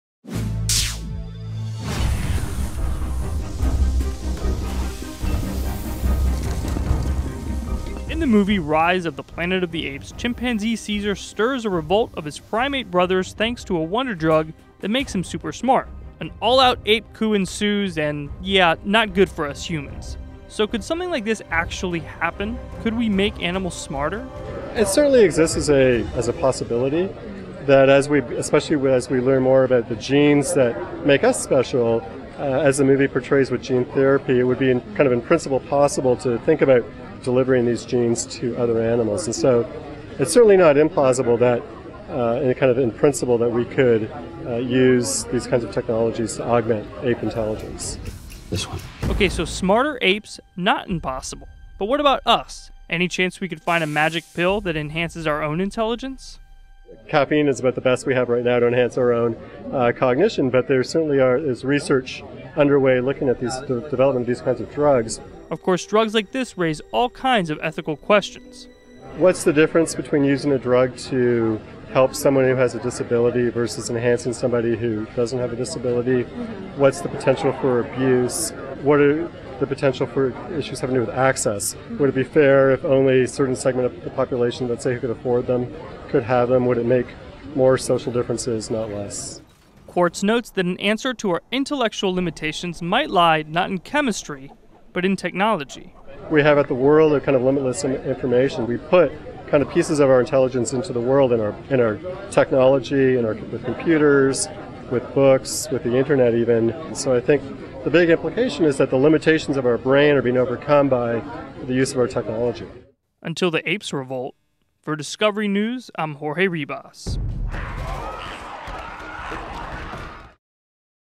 Rise of the Planet of the Apes: Can Drugs Make Us Smarter?
In the new movie Rise of the Planet of the Apes, Caesar the chimp leads a primate revolution thanks to a drug that makes him super smart. So when can we expect a pill for enhanced intelligence? Jorge Ribas finds out.